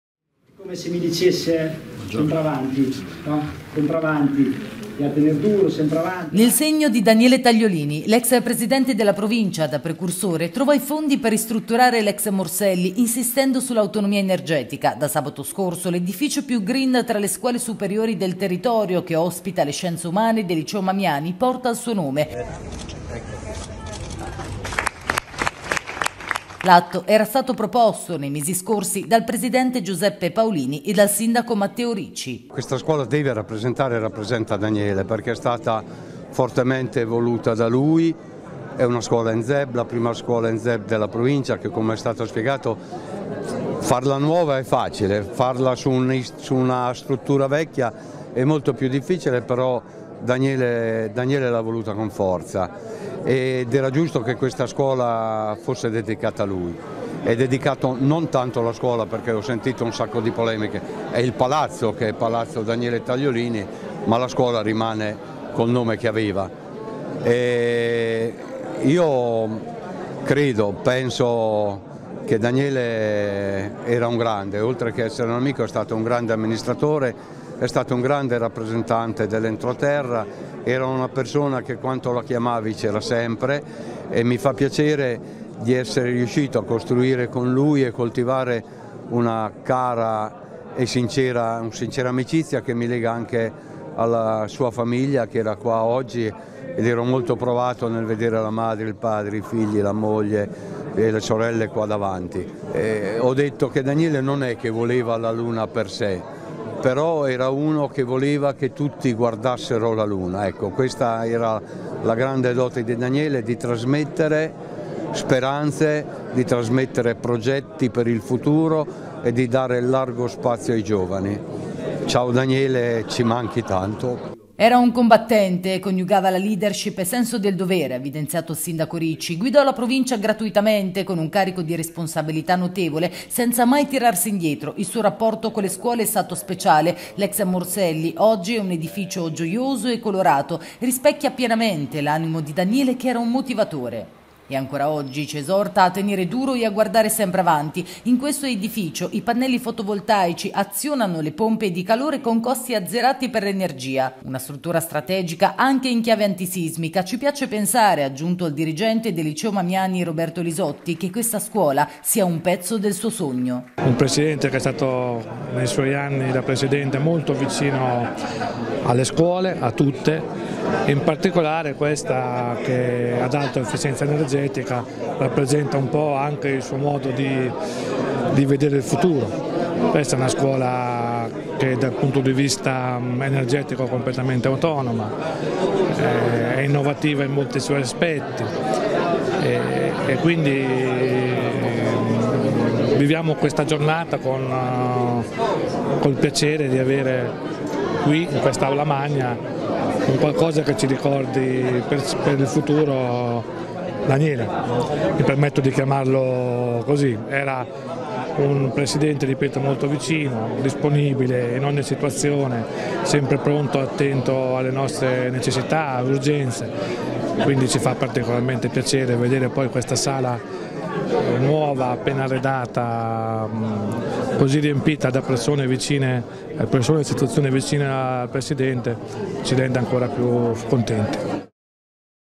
L'ex Morselli nel nome di Daniele Tagliolini
Sabato scorso a Pesaro si è svolta la cerimonia di intitolazione dell’edificio scolastico ex Morselli a Daniele Tagliolini, l’ex presidente della Provincia e sindaco di Peglio, scomparso poco più di un anno fa.